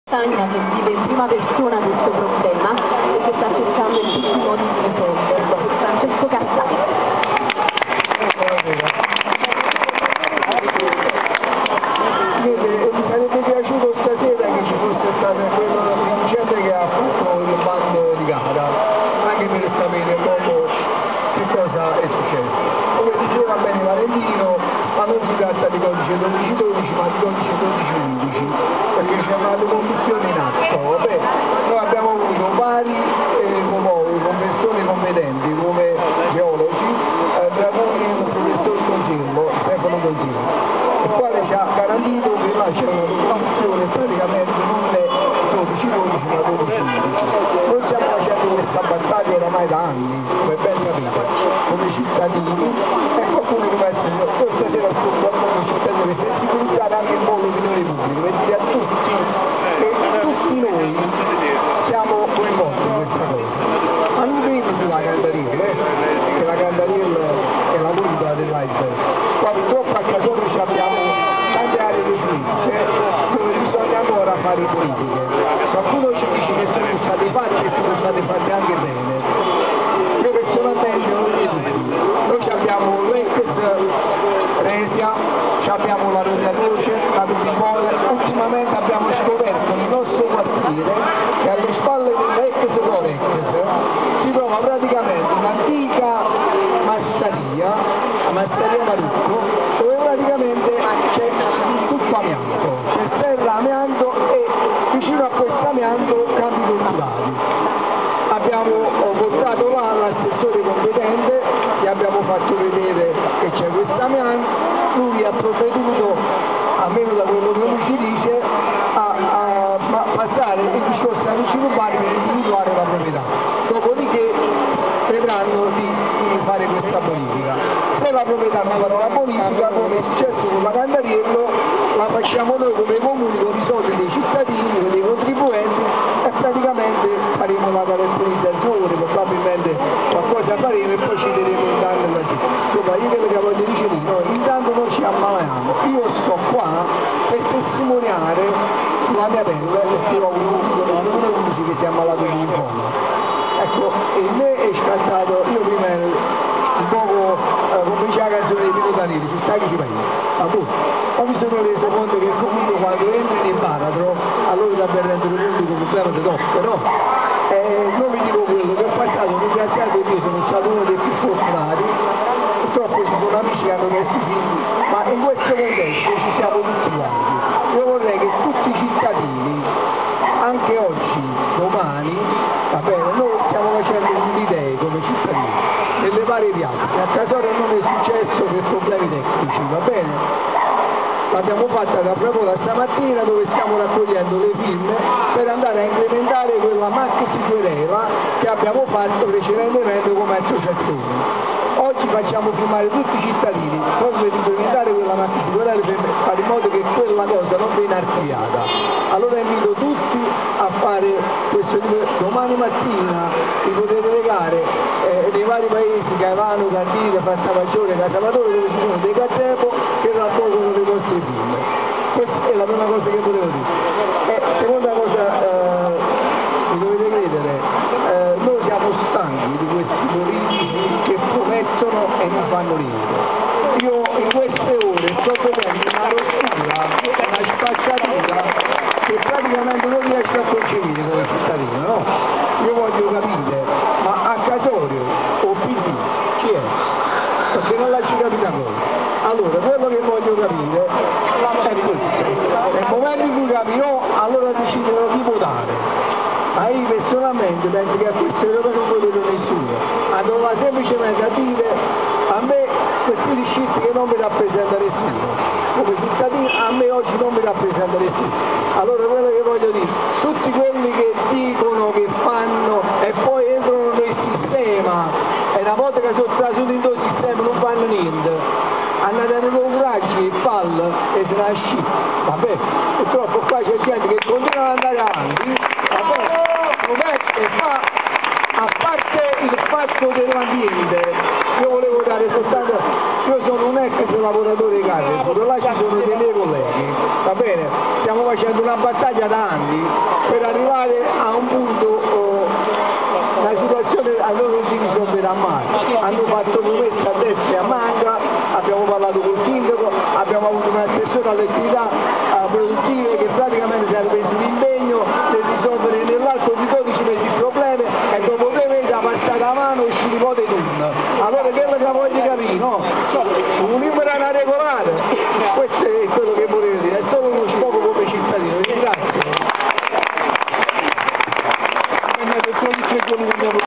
Casoria 17/05/2014 - Assemblea Pubblica:"Discarica Cantariello" (05)
"Casoria Ieri & Oggi online" presenta:
Casoria sabato 17 Maggio 2014 alle 18:00, in piazza San Paolo, l'Assemblea Pubblica promossa dai Giovani Democratici dell'Area Nord di Napoli (Casoria, Afragola, Casavatore, San Pietro a Patierno) sul tema della discarica sita in località Cantariello. Quello che doveva essere un sito temporaneo di stoccaggio dei rifiuti è diventato una vera e propria catastrofe ambientale, presente sul territorio da oltre dieci anni.